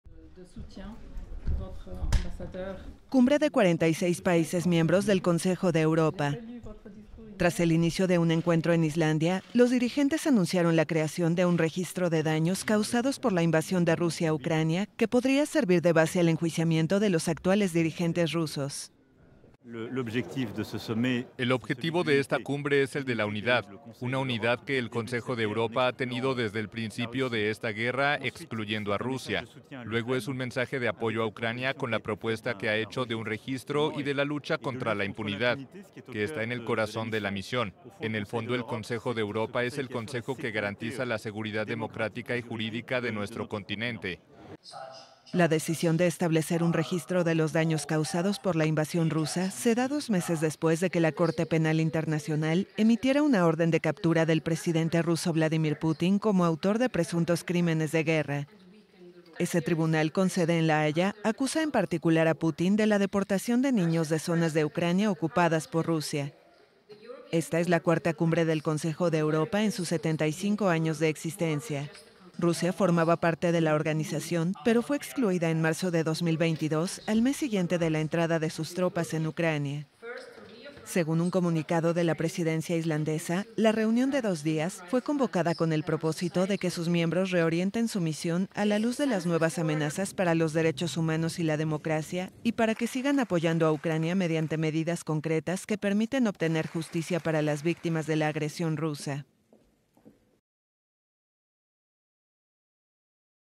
Europeos sientan bases para enjuiciar a líderes rusos por invasión a Ucrania
Los dirigentes de los 46 países miembros del Consejo de Europa crearon el martes un "registro de daños" causados por la invasión rusa de Ucrania, que podría servir de base al enjuiciamiento de los actuales dirigentes rusos.